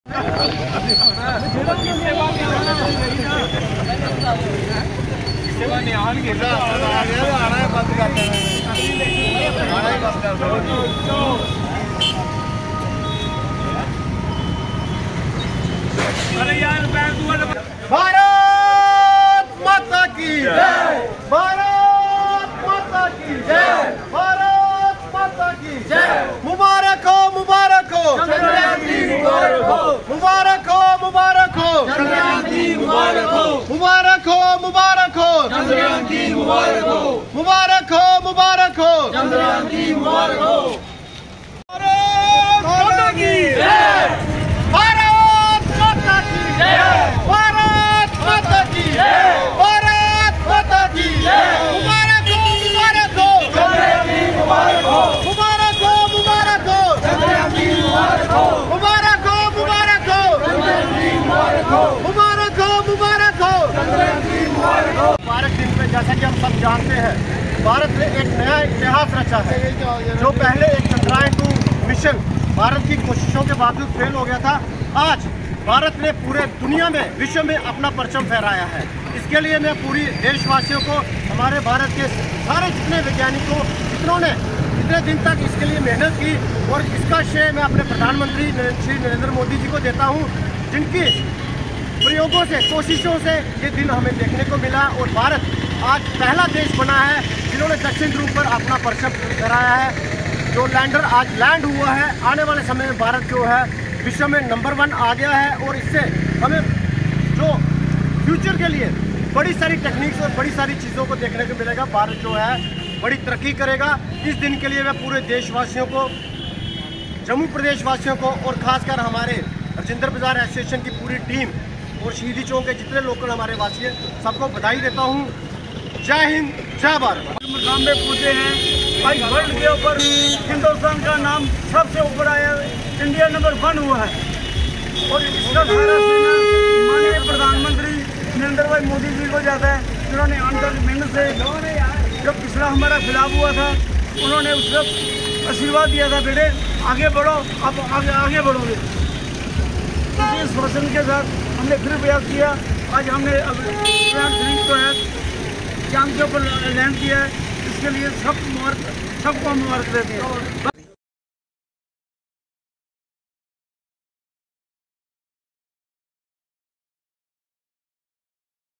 Celebrations at Shahidi Chowk Jammu by Locals and Rajinder Bazar Association over successful landing
Celebrations at Shahidi Chowk Jammu by Locals and Rajinder Bazar Association over successful landing of Chandrayaan3 on Moon. Congratulations to Every Indian on this Historical day.